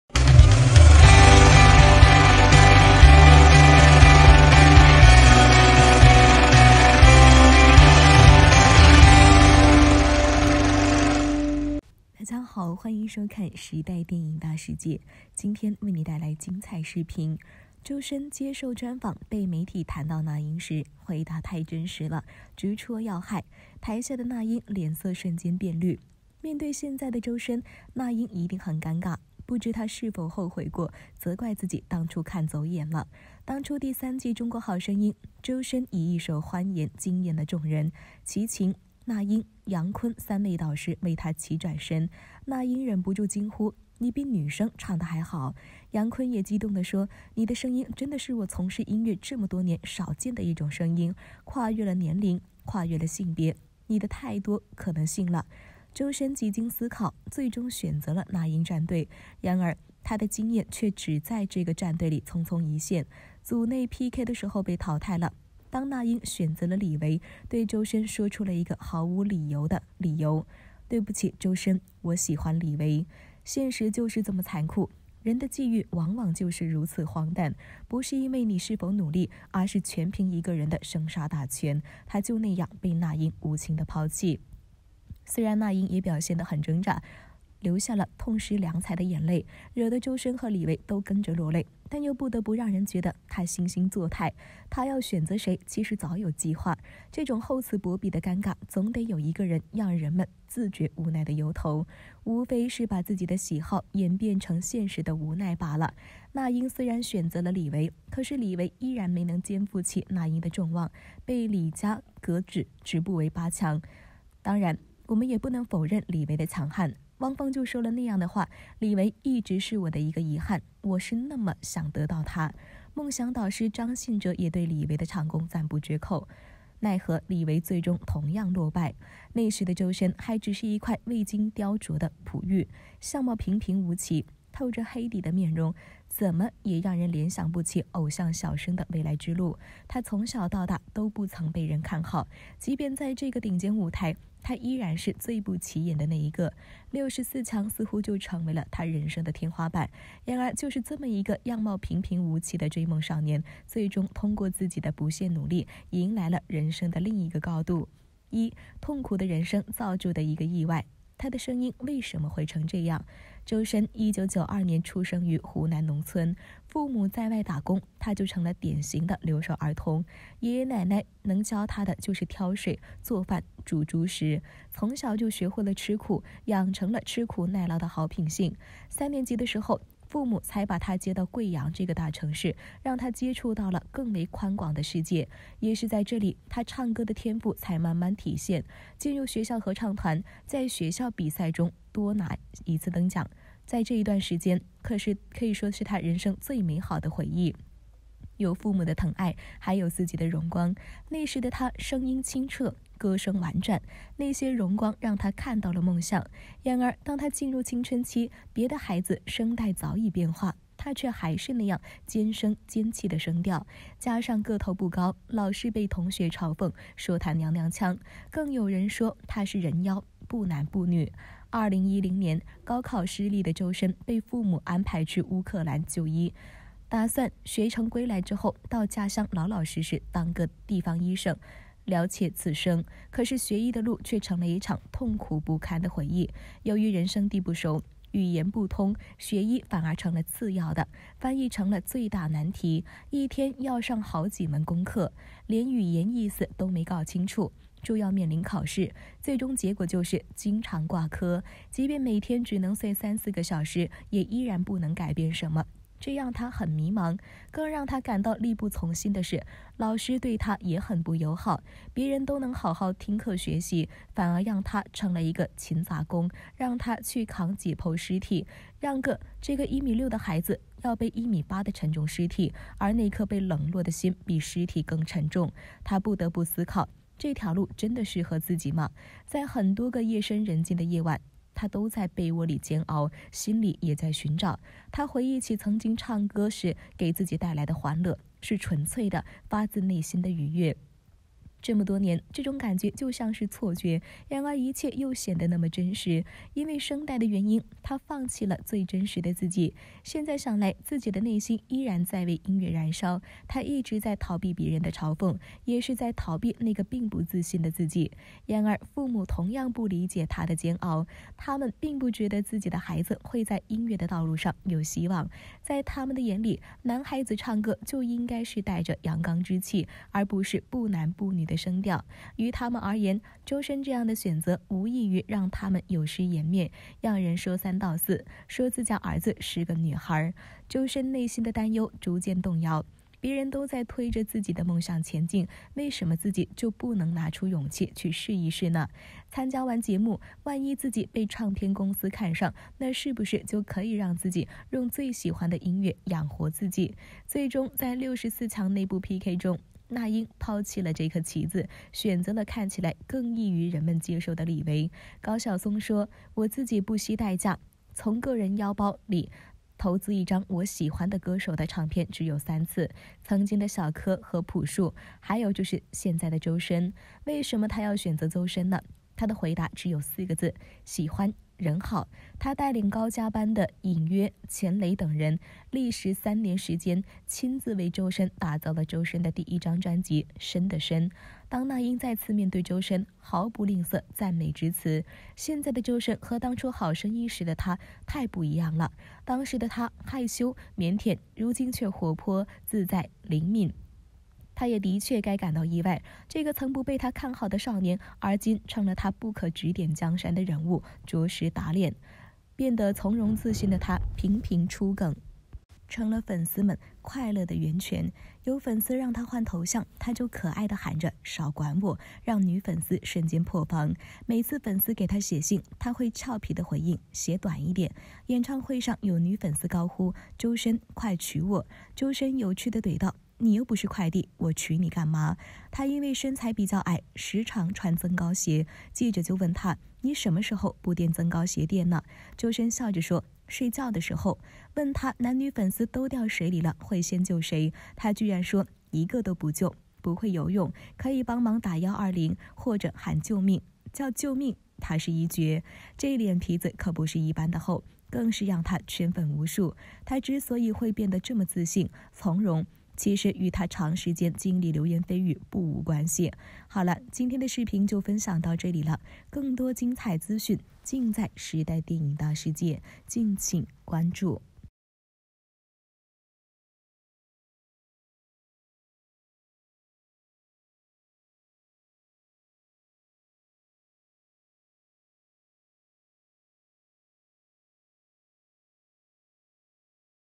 周深接受專訪被媒體談到那英時，回答太真實了，直戳要害，臺下的那英臉色瞬間大变，現場甩手離開！時代電影大世界
周深接受專訪被媒體談到那英時，回答太真實了，直戳要害，臺下的那英臉色瞬間大变，現場甩手離開！時代電影大世界#周深#那英